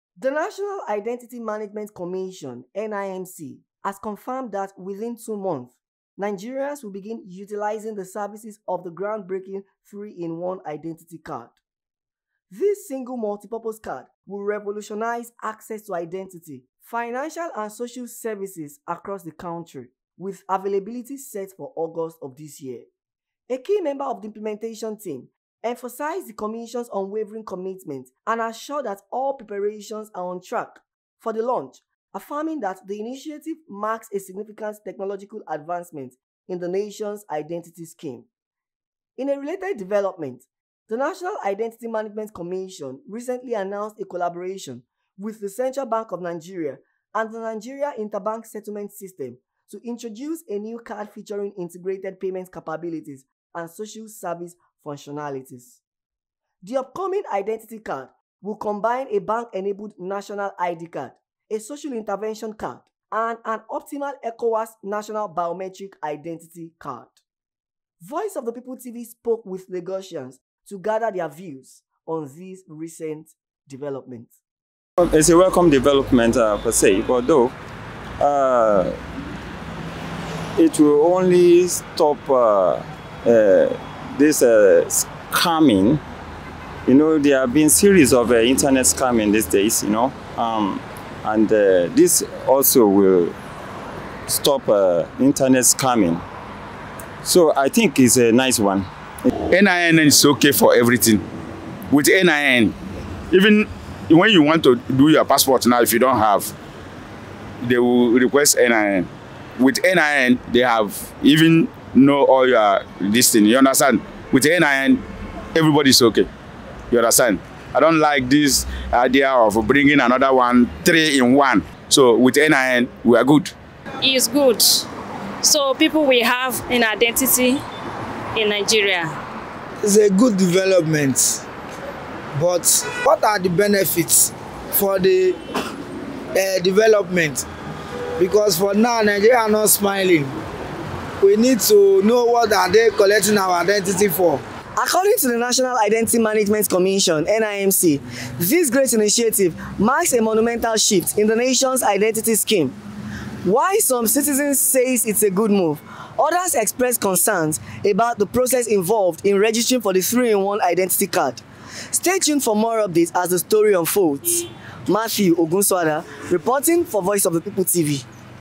NEW IDENTITY SCHEME : NIMC ANNOUNCES THREE-IN-ONE IDENTITY CARD SET TO LAUNCH BY AUGUST
Voice of the People TV, Nigeria